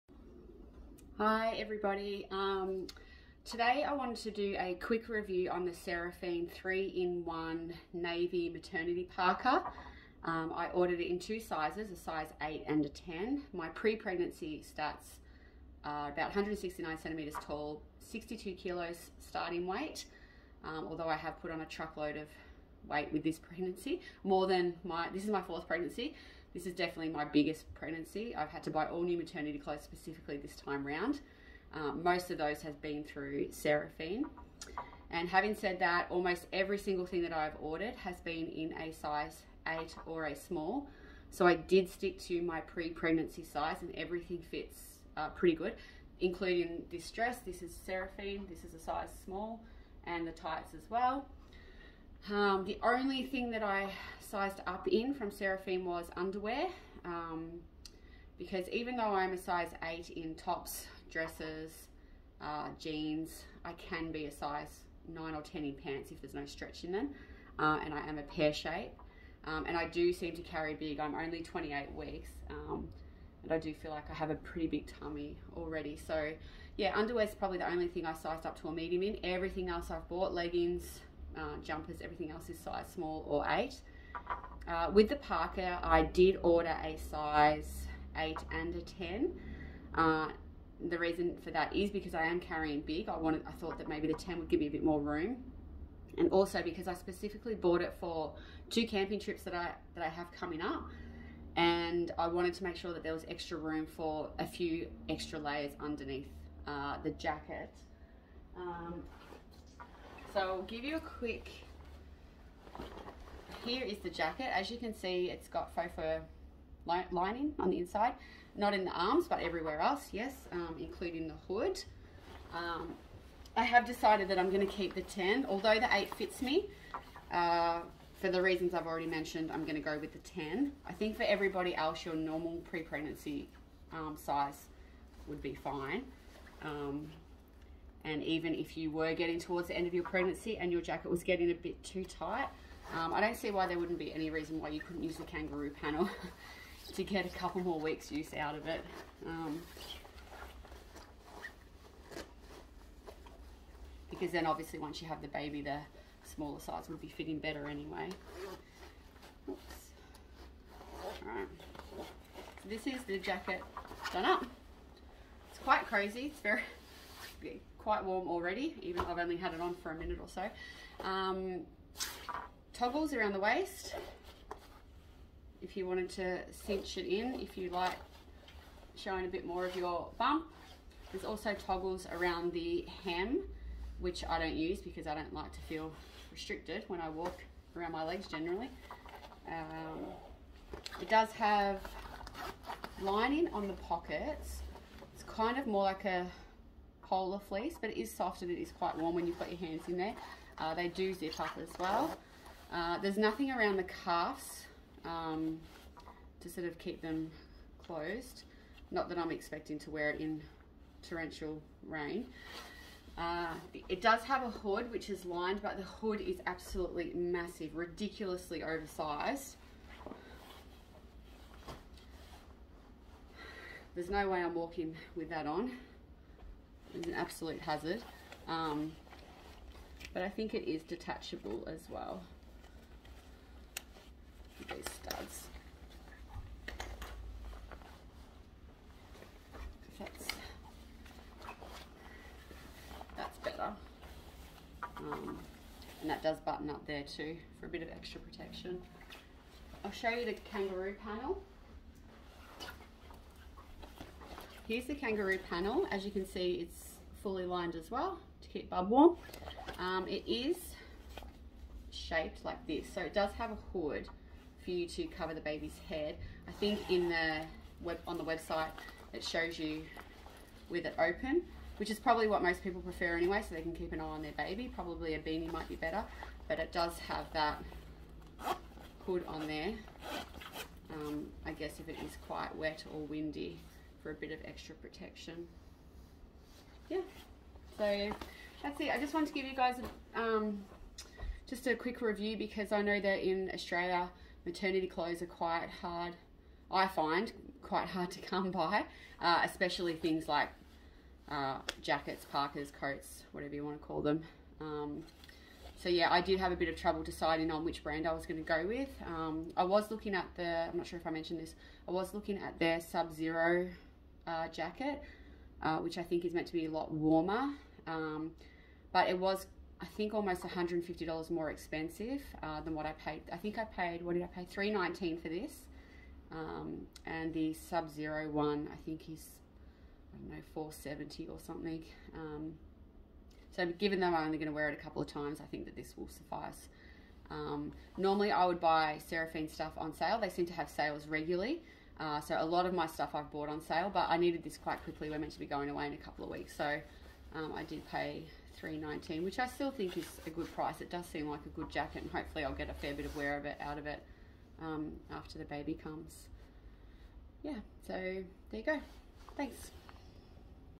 seraphine 3 in 1 maternity parka review
pre-pregnancy stats
169cms
62kgs
Size S or 8 AU in almost everything, except  pants (9 or 10)

Pregnancy stats: 28 weeks here and 77kgs (yes a lot I know lol)
Everything I have in Seraphine: tops, tunics, jumpers, leggings all size S or 8, except underwear (M)
The size 8 jacket did fit, but kept the 10 as I wanted extra room as I have put on a lot of weight with this pregnancy, and wanted the extra room for multiple layers underneath when camping.